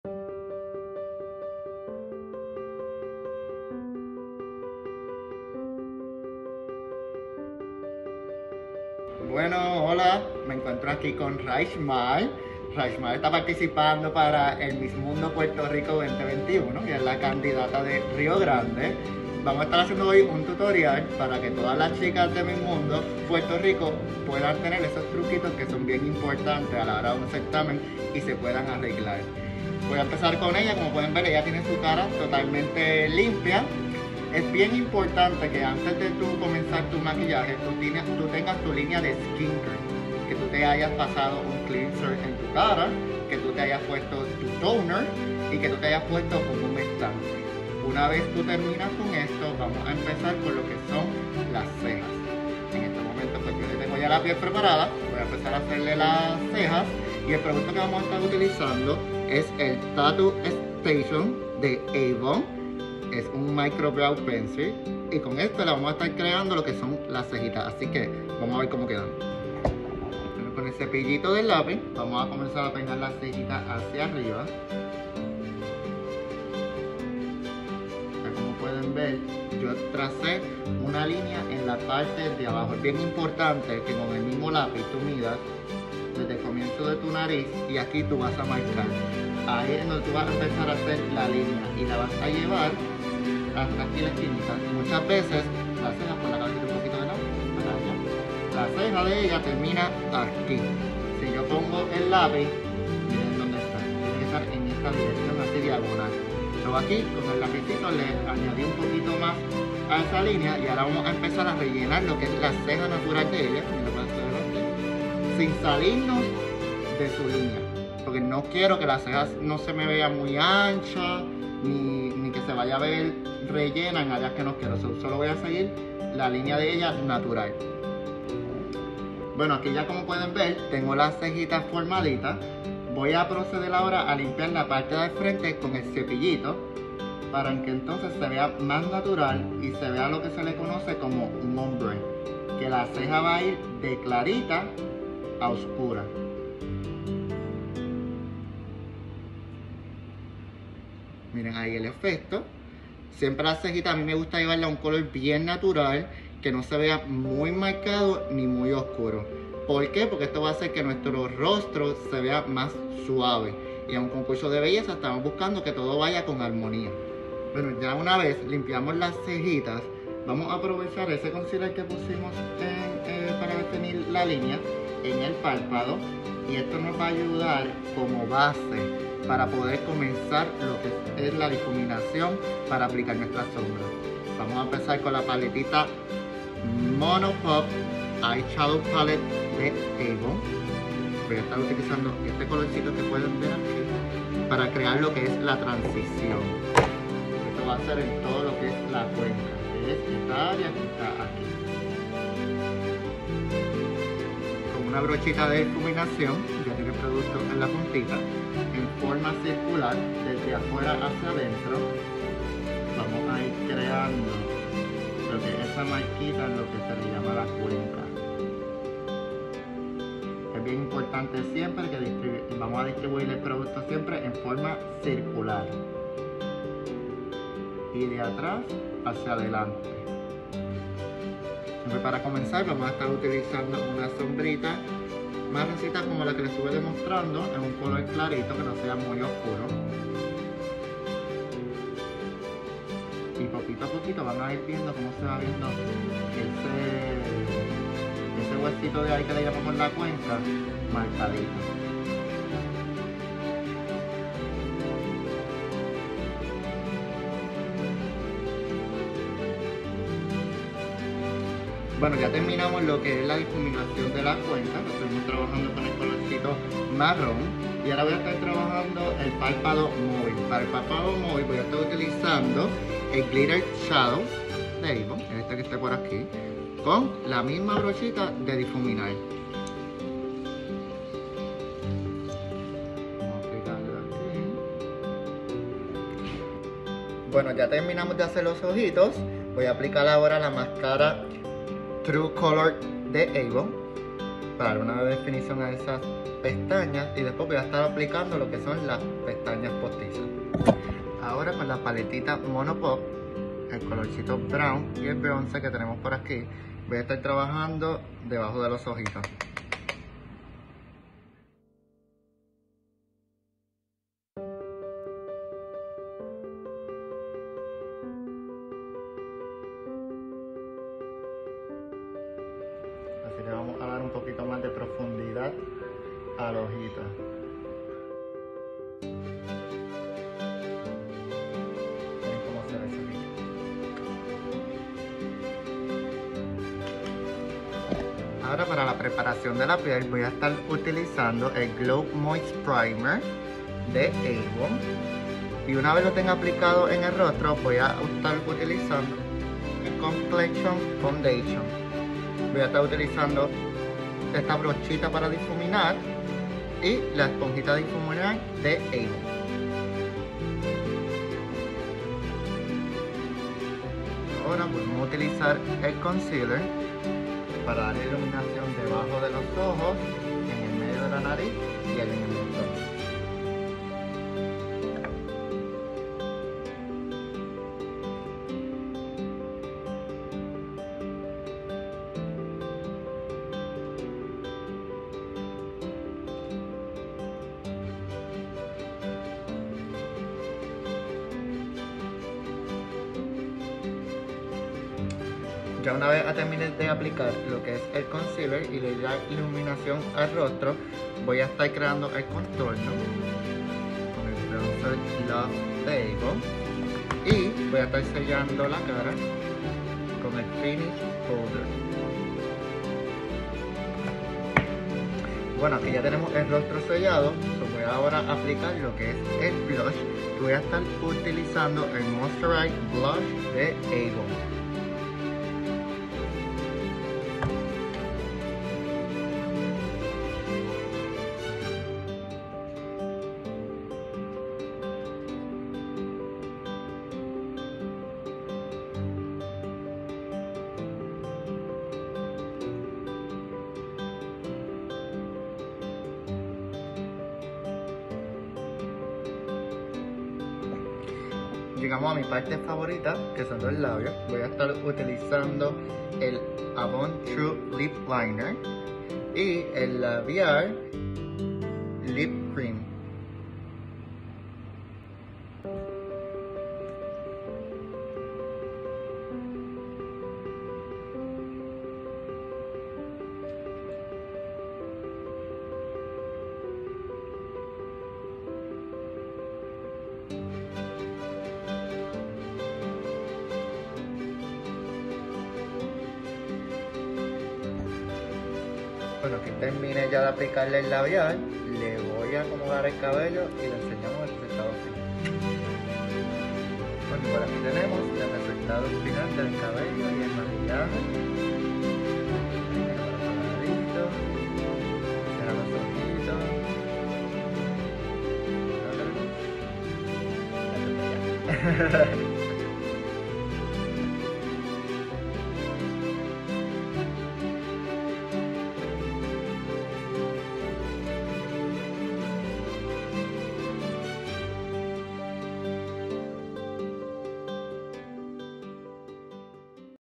Primer Seminario de Maquillaje Avon by Louis Ernest for MMPR 2021 1
Avon presenta al Makeup artist Louis Ernest en Primer Seminario de Maquillaje a las candidatas a Miss Mundo de Puerto Rico 2021.